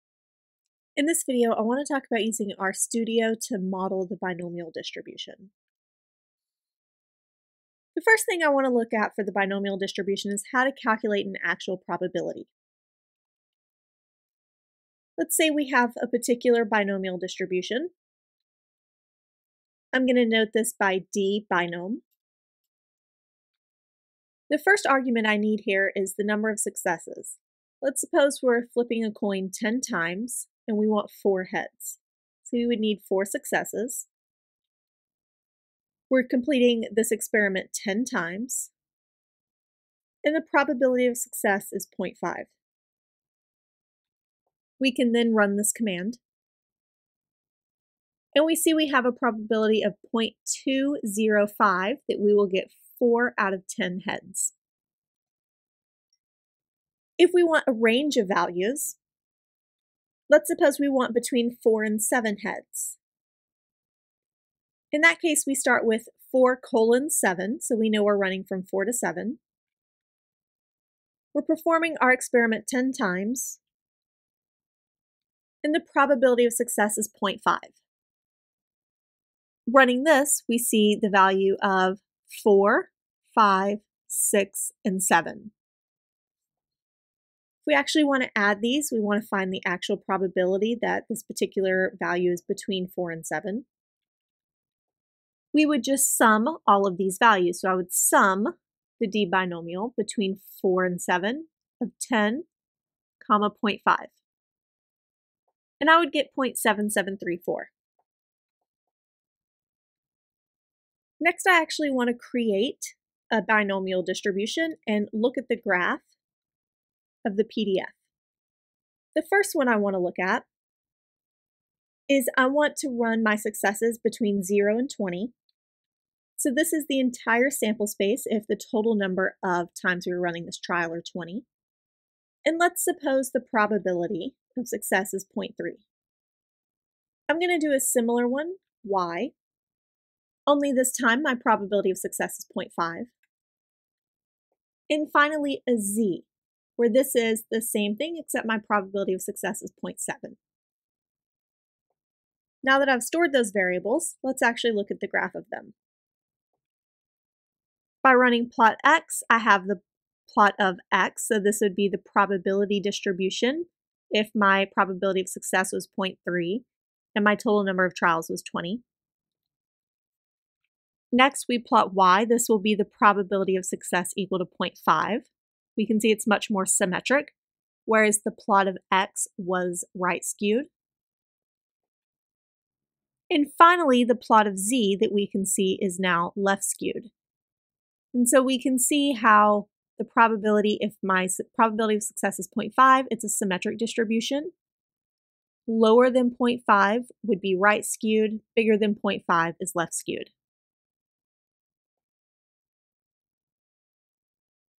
Binomial Distributions in R
I show how to use R Studio to calculate binomial probabilities. I also show how to create the graph of a PMF for a binomial distribution, and discuss what these look like for different values of p.